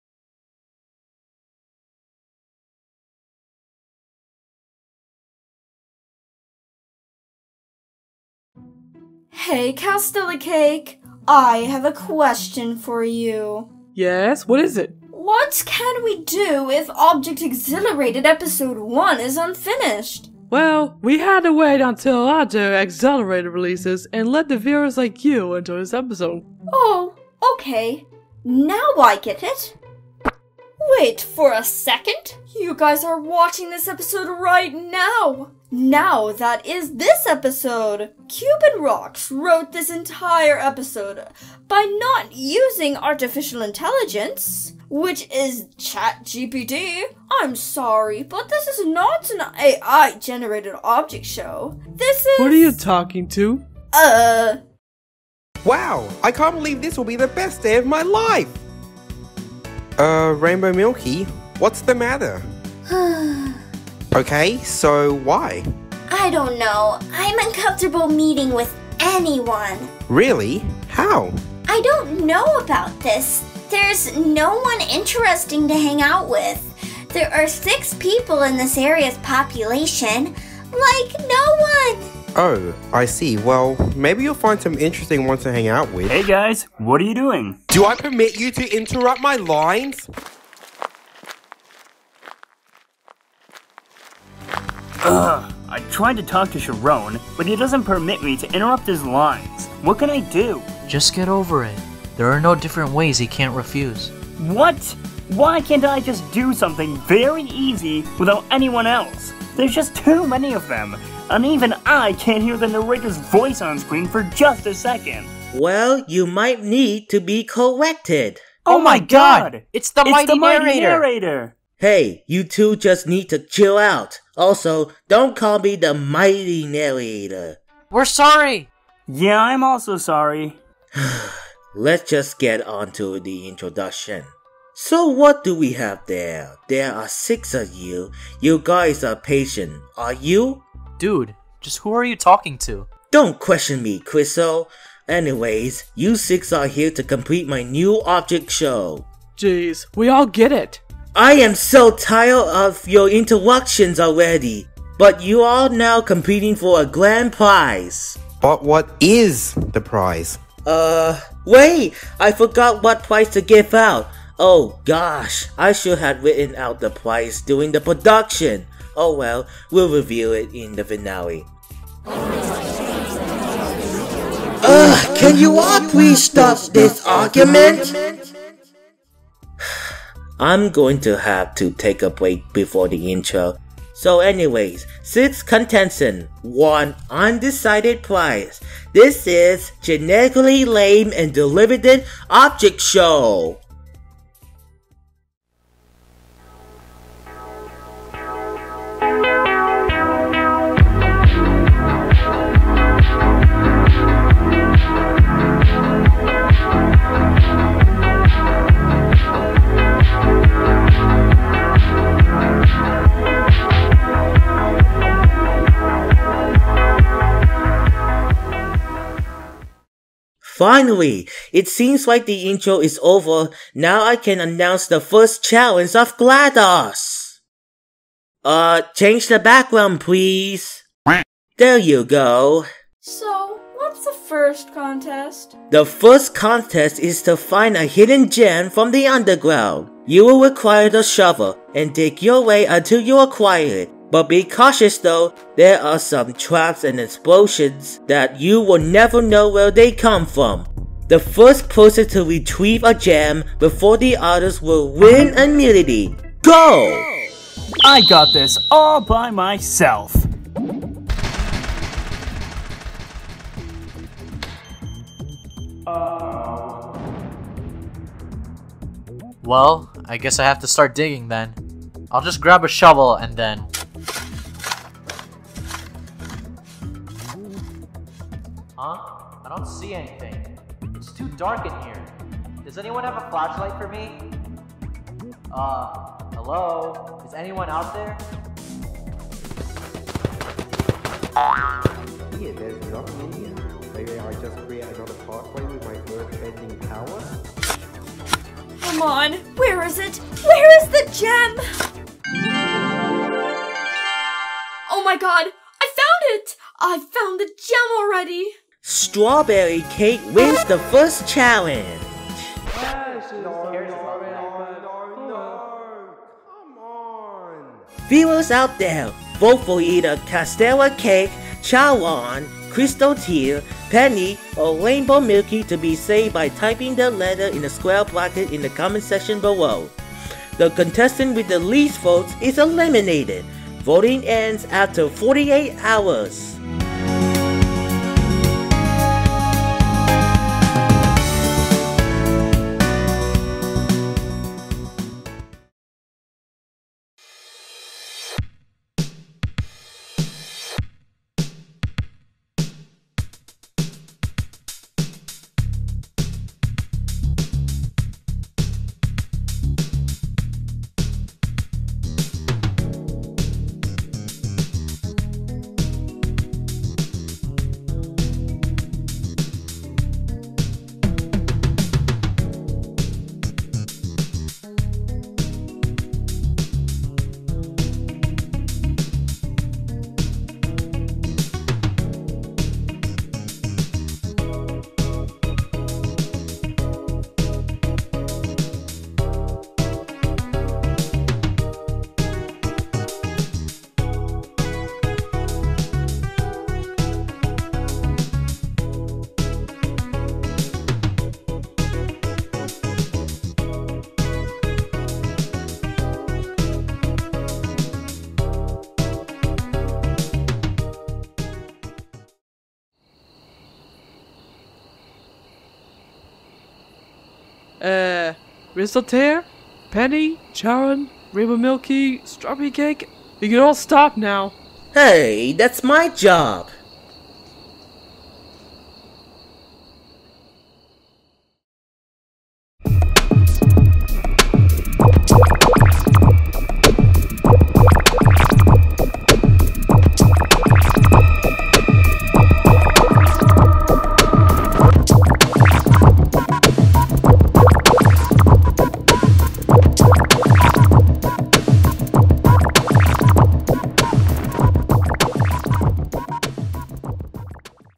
GLADOS 1: "The Episode"
Somewhere in the green fields, 6 contestants are competing for... WHAT PRIZE?! What do you mean the prize is undecided?! For what reason? Oh well. They must do the first challenge by digging themselves to achieve the gem before anyone else! But perhaps, enjoy the show!

A show inspired by The Daily Object Show by FusionAnimations, Twisted Turns Reboot by TardistheTardis, Victory Will Be Mine by Cole The Geek, and Paper Puppets Take 2 by YellowAngiru.

generically lame and derivative object show episode 1